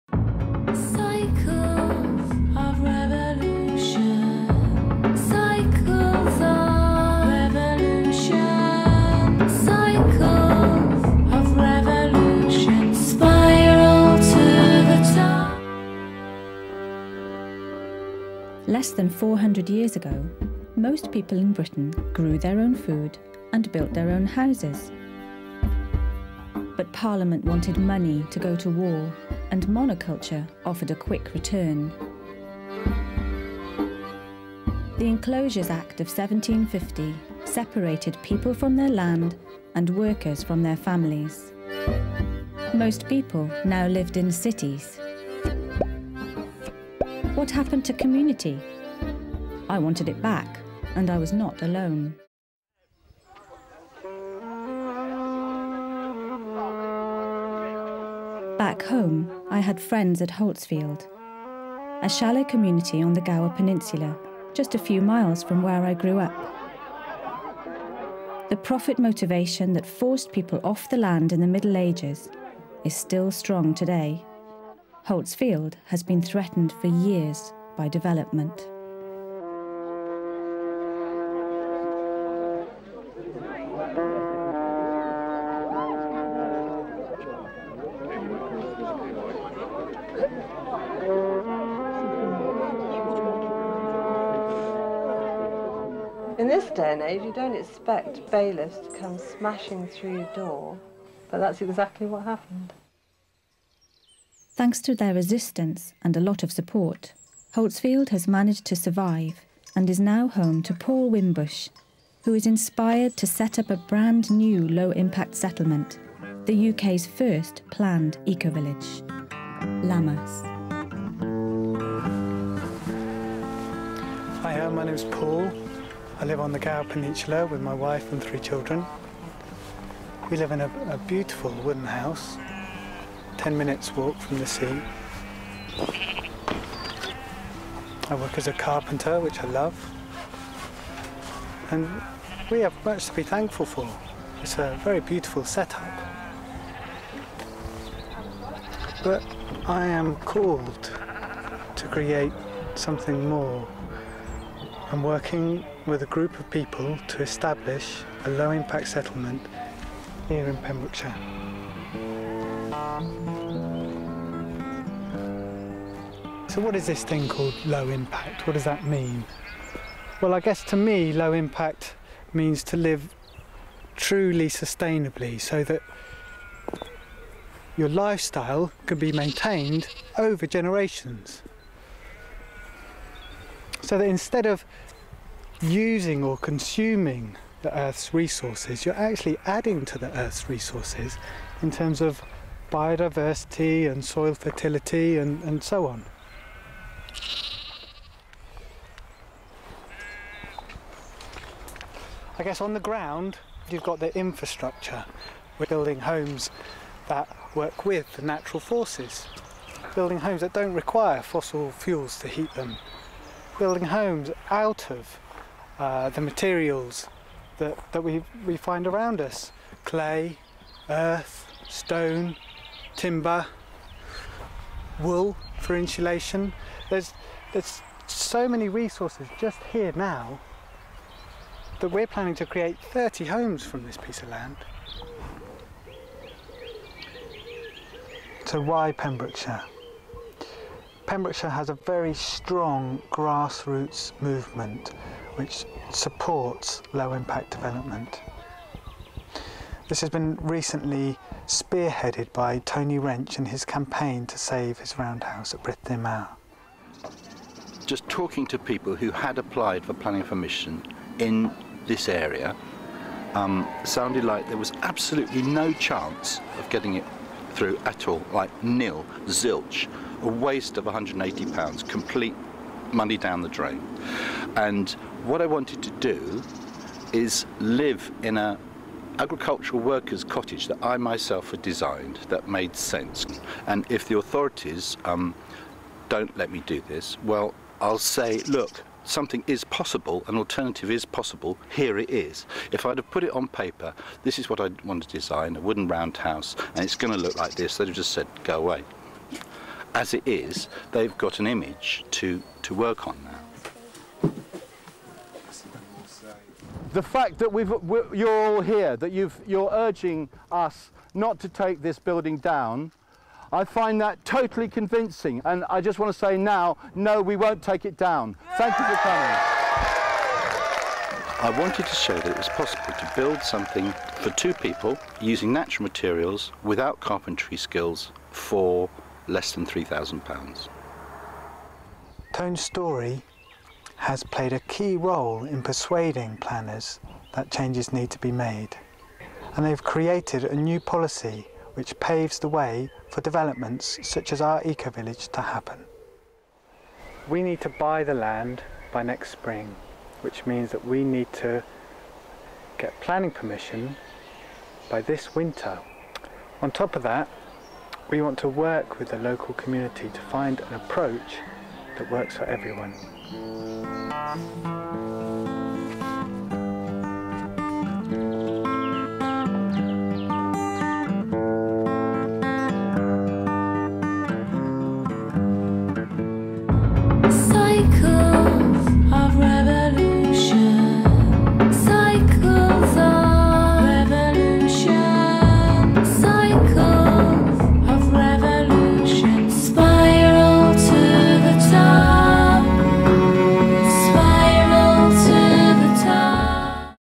Living in the Future Episode 1: Introducing Lammas
Lammas is the UK's first planned ecovillage. It is designed under Pembrokeshire Authority's innovative Policy 52, which allows for sustainable development in the countryside. In Britain, no development has been permitted in the countryside since world war 2. The plans are for a new settlement of 9 eco-smallholdings, a campsite and a community hub building. It is sited on 76 acres of mixed pasture and woodland next to the village of Glandwr, Pembrokeshire.